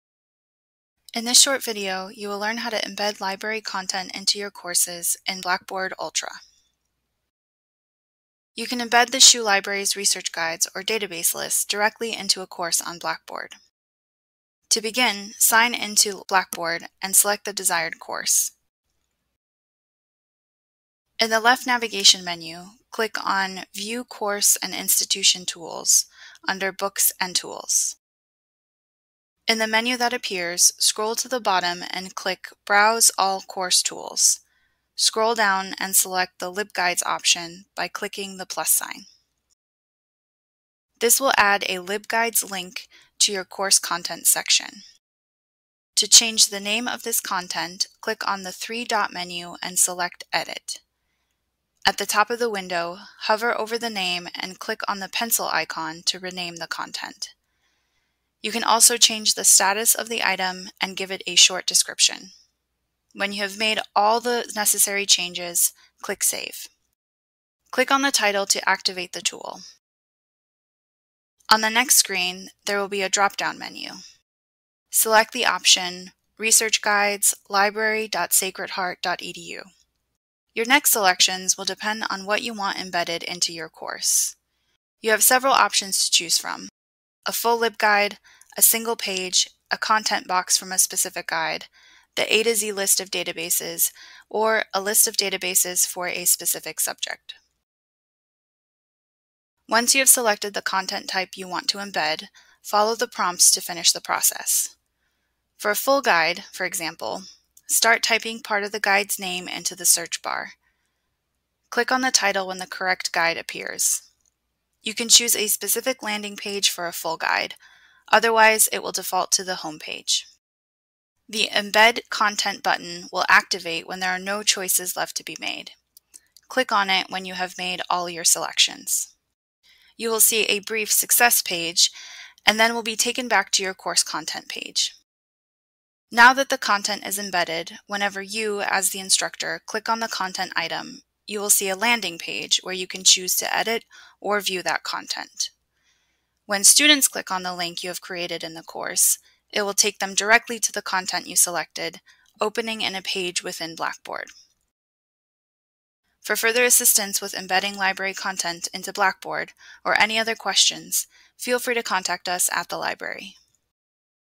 Embedding Library Content in Blackboard Ultra
In this short video, you will learn how to embed library content into your courses in Blackboard Ultra.